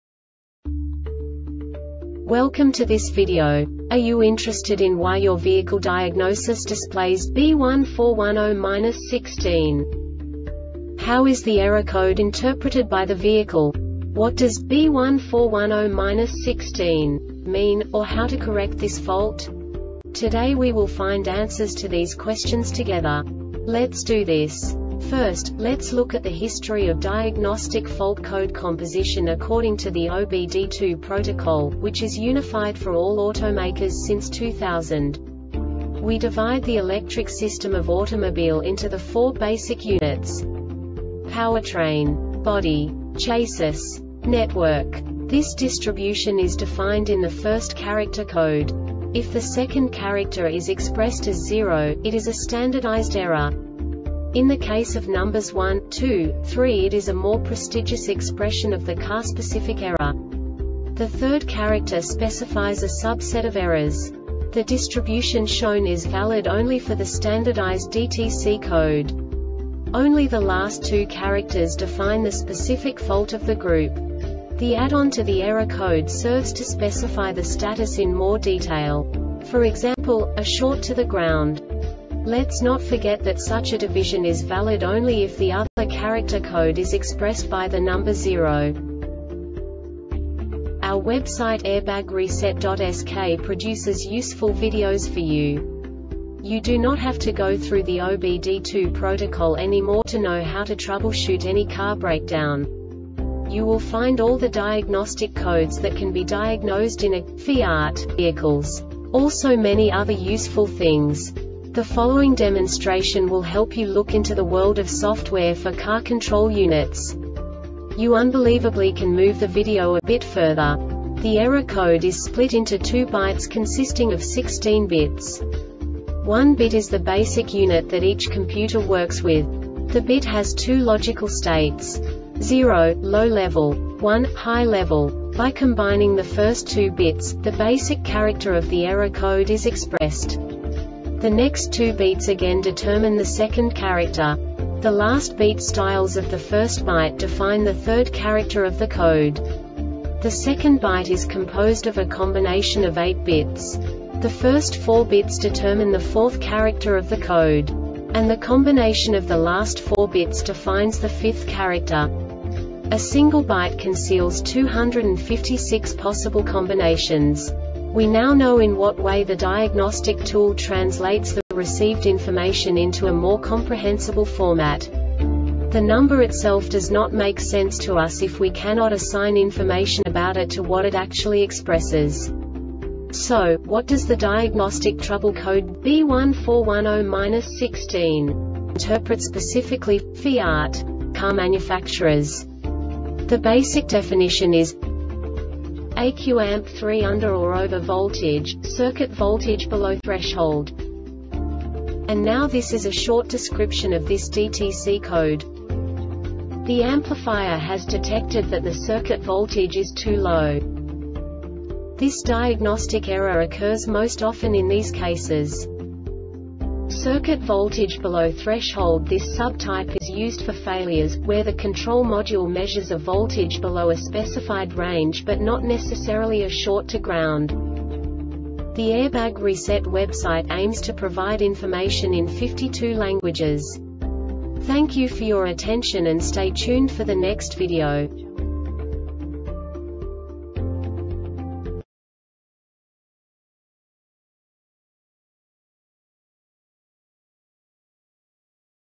DTC Fiat B1410-16 Short Explanation
Contents:
0:21 Basic DTC analysis according to OBD2 protocol standard.
1:48 Insight into programming
2:58 A diagnostically understandable description of the Diagnostic Trouble Code
3:05 Basic error definition

Make:
FIAT
Code:
B1410-16
Definition:
Ecu Amp 3 (Under or Over Voltage) - Circuit Voltage Below Threshold
Description:
The Amplifier has detected that the circuit voltage is too low.